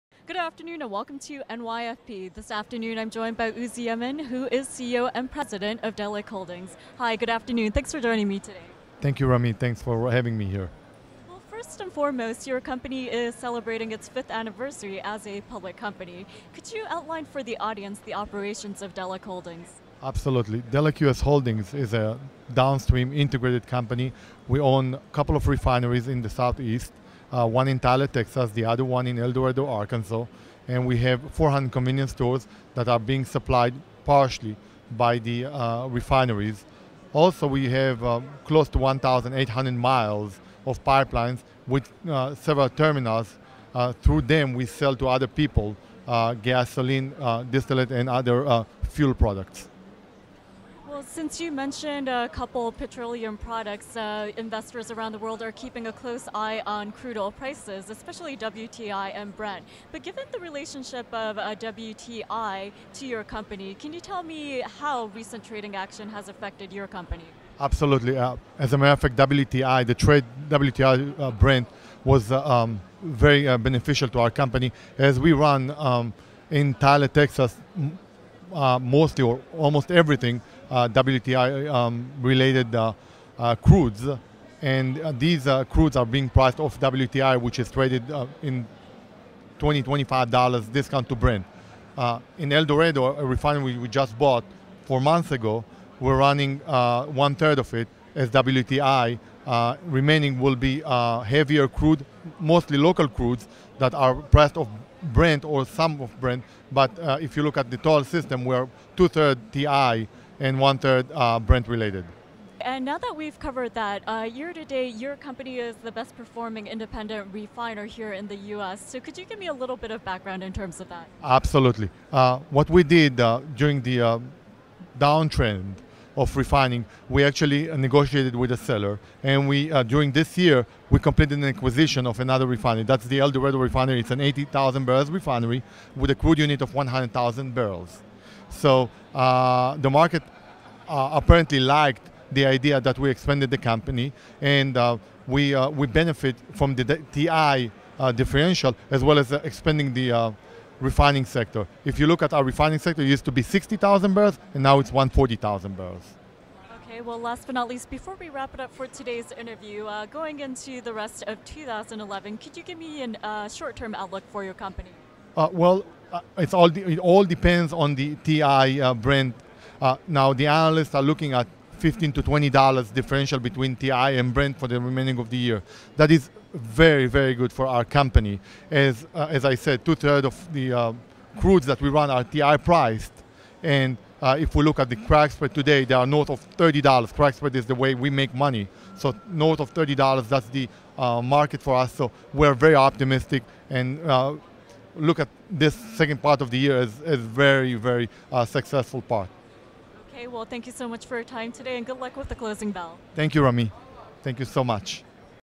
CEO Spotlight: Uzi Yemin, CEO and President of Delek Holdings
08/23/2011 Delek Holdings' Uzi Yemen discusses its recent acquisition of a major equity interest in Lion Oil Company and near-term outlook for the company.